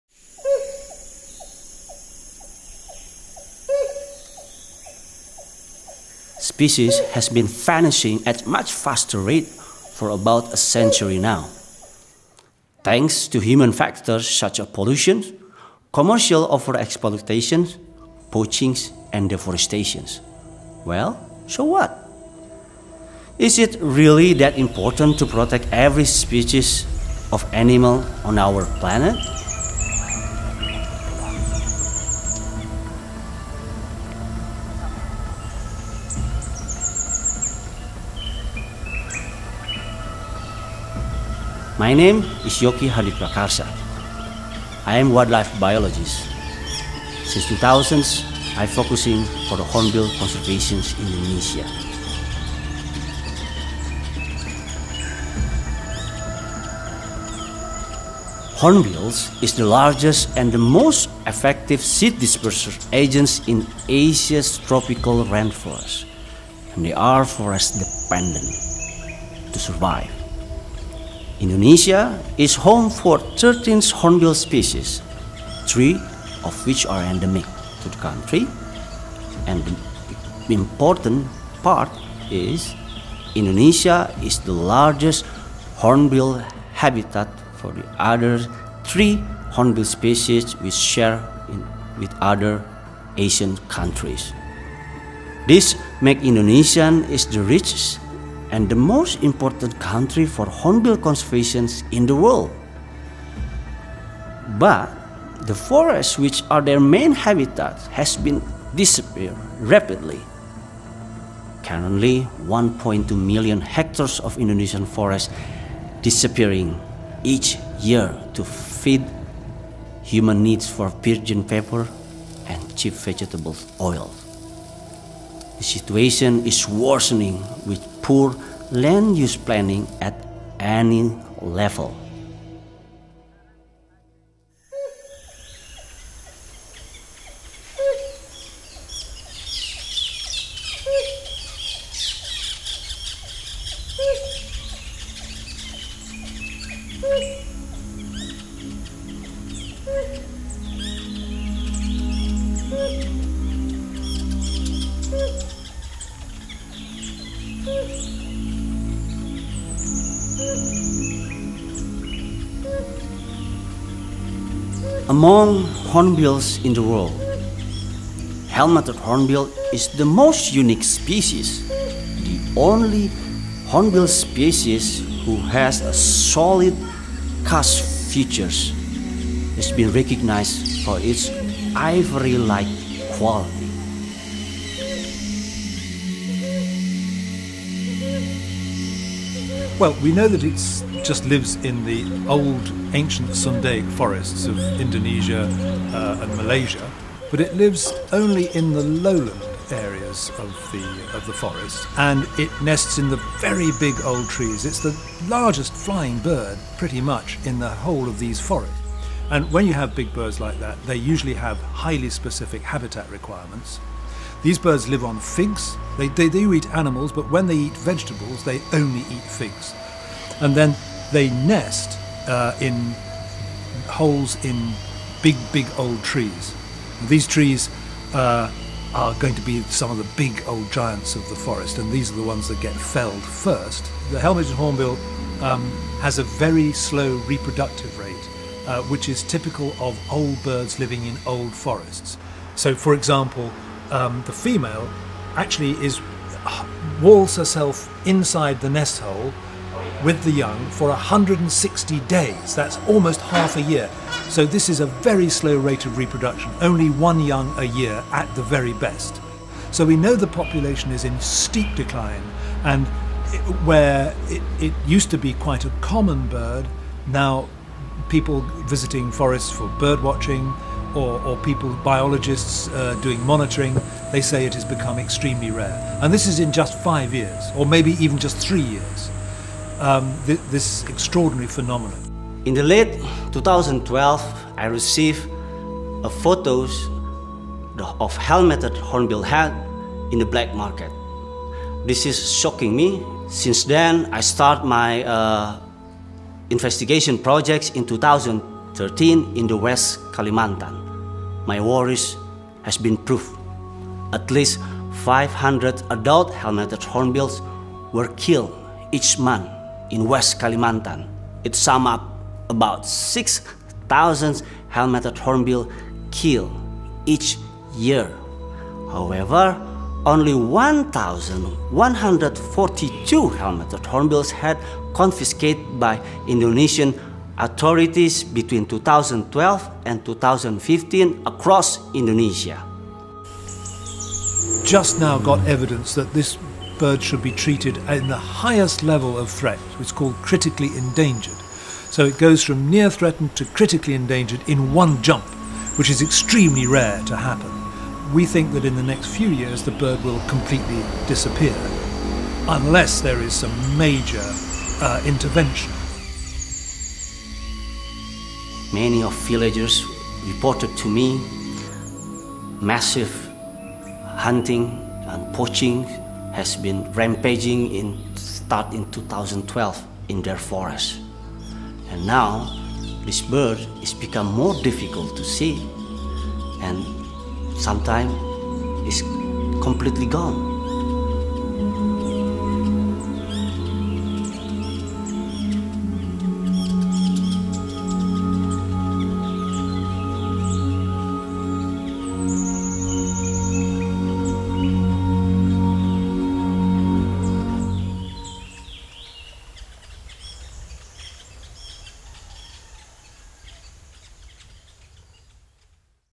Call From Majestic Bird
Indonesia is home to thirteen hornbill species, three of which are endemic. This makes Indonesia the richest and the most important country for hornbill conservation in the world.

Hornbill is the largest and most effective seed disperser agent in Asia's tropical rainforests and they are forest dependent. Among Asian hornbills, the Helmeted hornbill is the most unique species starting from species life requirement and features a solid casque that has been recognized for its ivory-like quality. It is the only Hornbill in the world who has this feature.